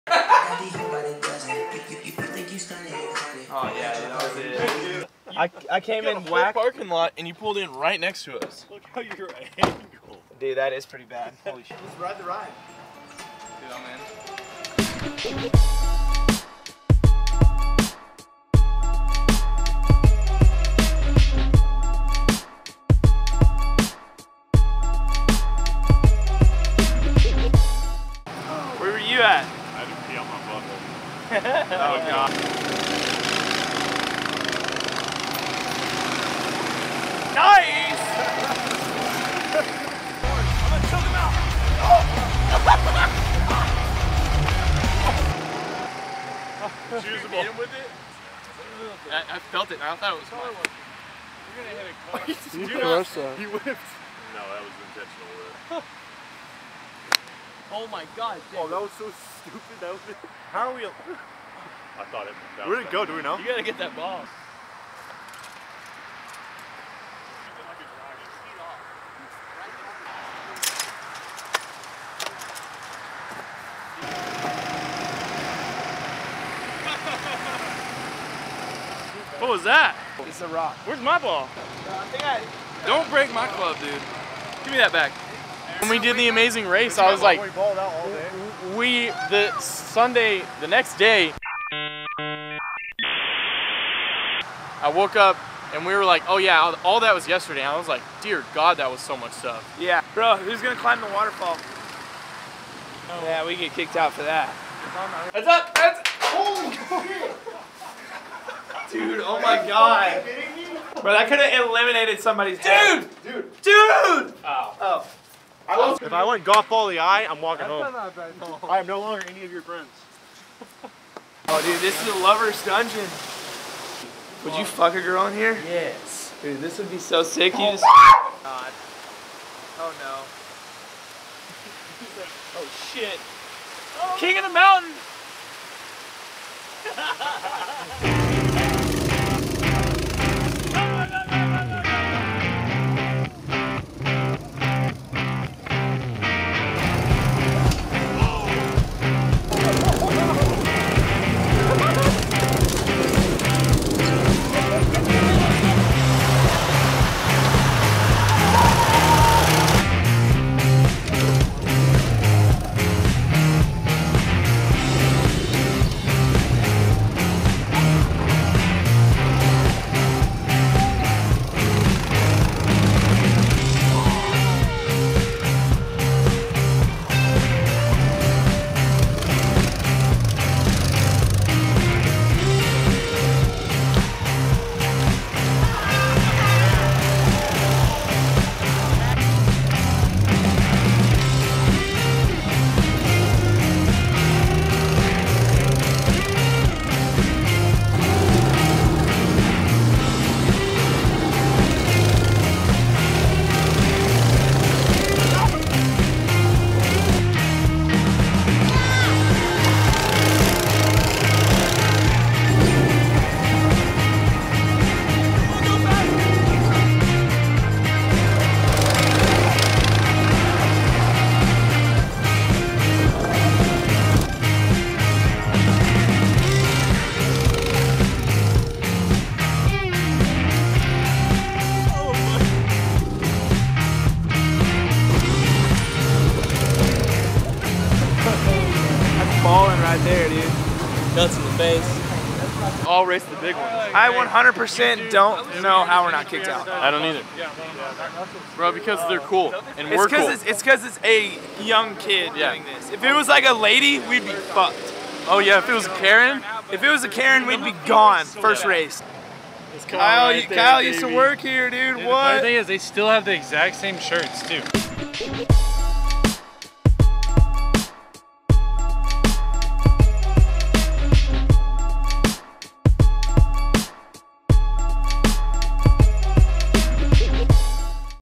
Getting kicked out is impossible
Clap Factory go karting turns into high speed bumper cars. Our game of mini golf goes south fast.